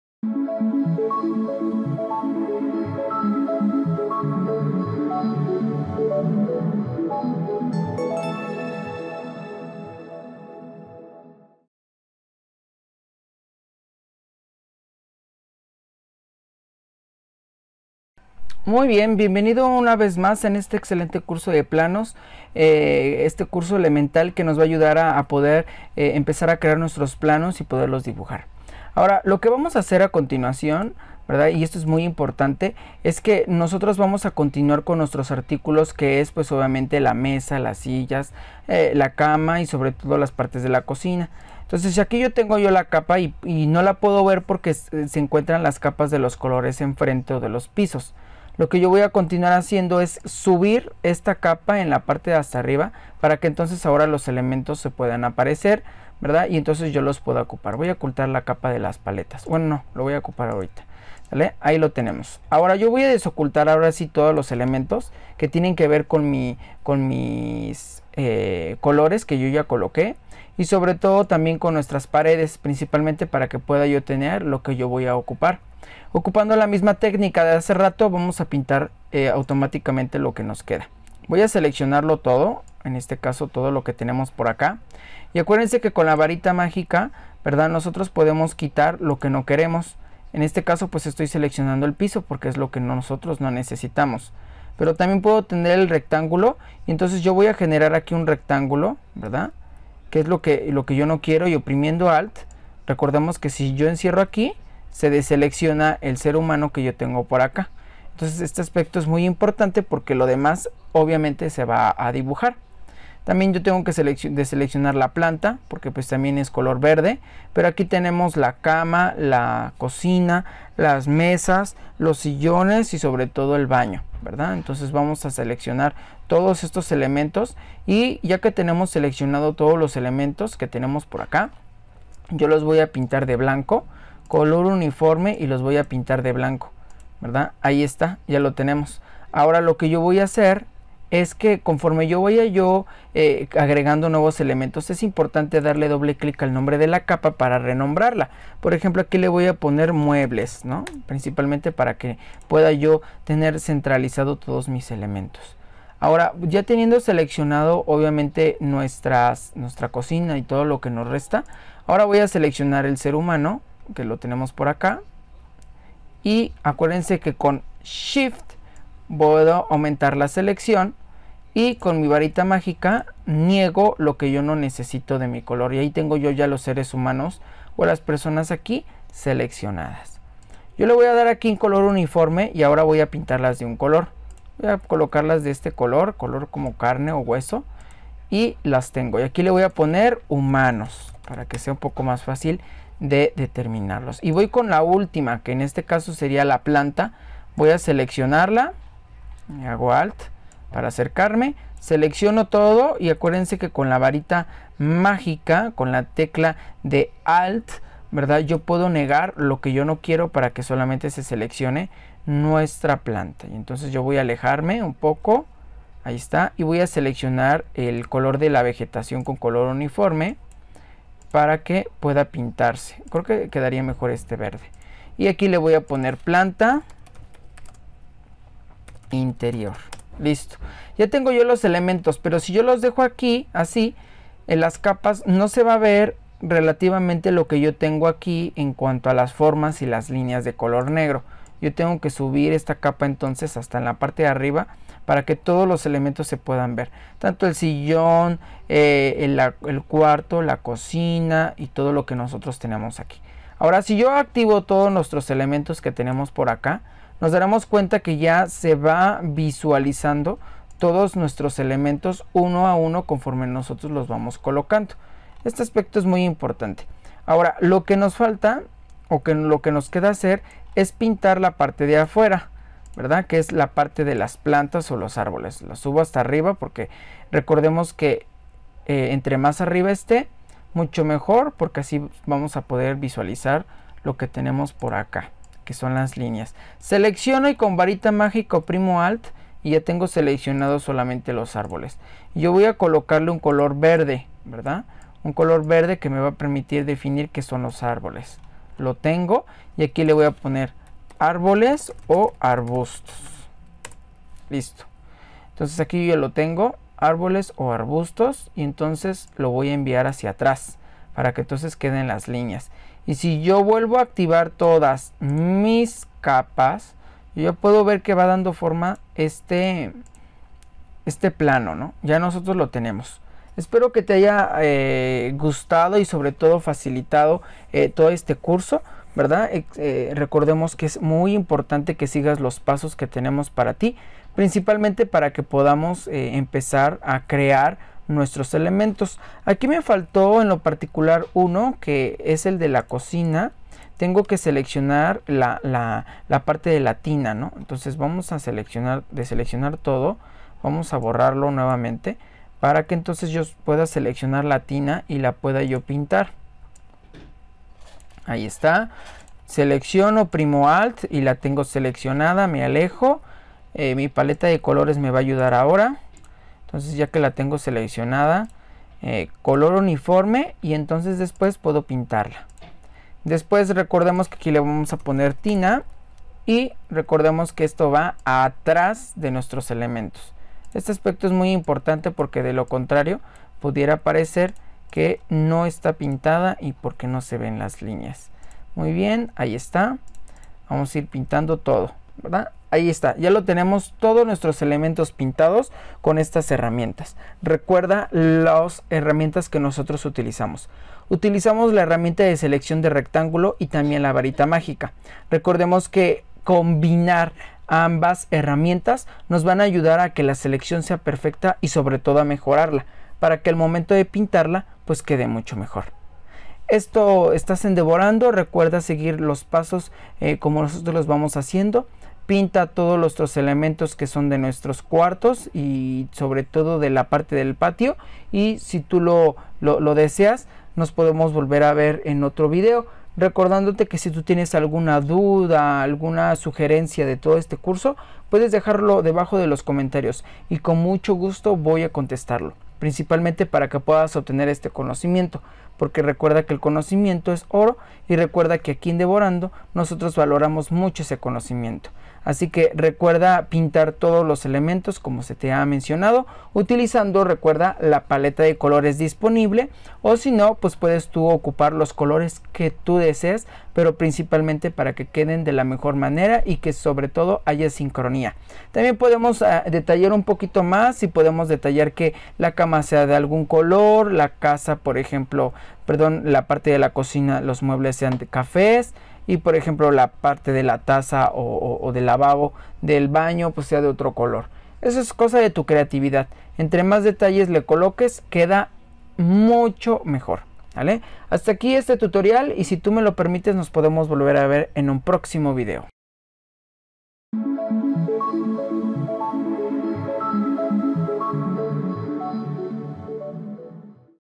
🏤 13 Colores 2 Curso para Planos ▶ Photoshop DEVORANDO DIPLOMADO GRATIS🖱️
✅ APRENDE este excelente tutorial y podrás a llegar a hacer diseño profesionales mediante esta excelente herramienta🤯. Conoce este proceso de construcción de crear PLANOS en Photoshop.

LEARN this excellent tutorial and you will be able to make professional designs using this excellent tool🤯. Get to know this process of building PLANS in Photoshop.

Correo para enviar Proyecto /Mail to send Project .

Happy design ;)
Feliz Diseño ;)